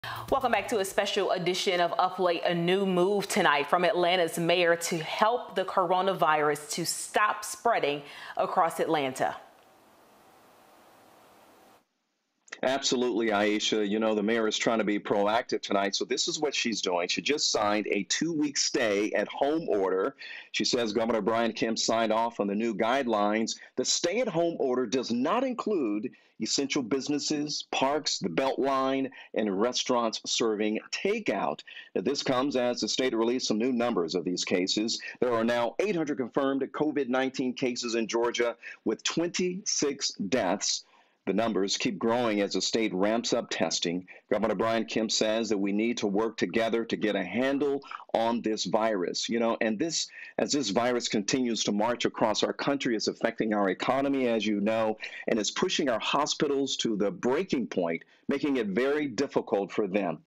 Atlanta Mayor Keisha Lance Bottoms issues 14-day stay-at-home order
She said the executive order does not not include essential businesses, parks, the Atlanta BeltLine or restaurants serving takeout.